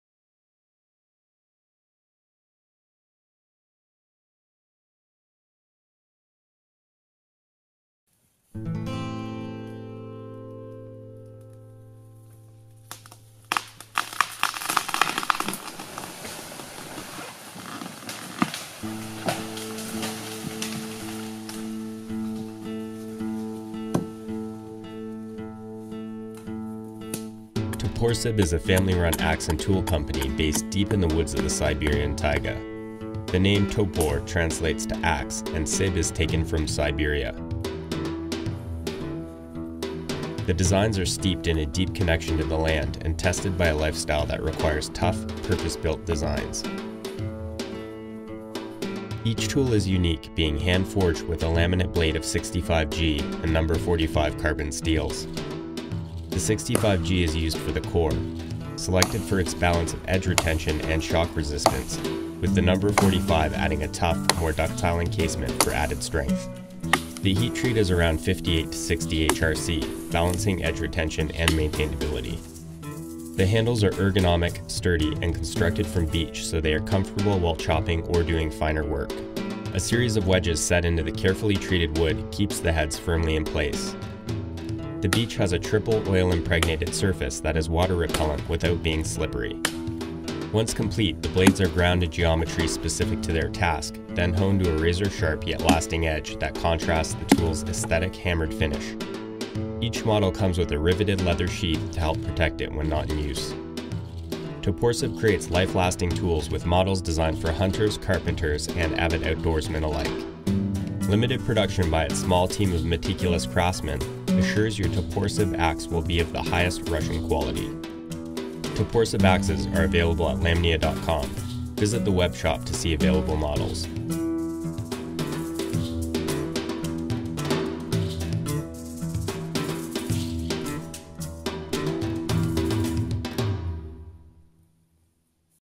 Магазин "Lamnia " . Для тех кто за границей .
Наши друзья из финляндии попросили выложить это видео и установить его на сайт .
Мы сами топоры за рубеж не высылаем . Купить топоры нашей кузницы за рубежом можно только в магазине Lamnia . Сайт магазина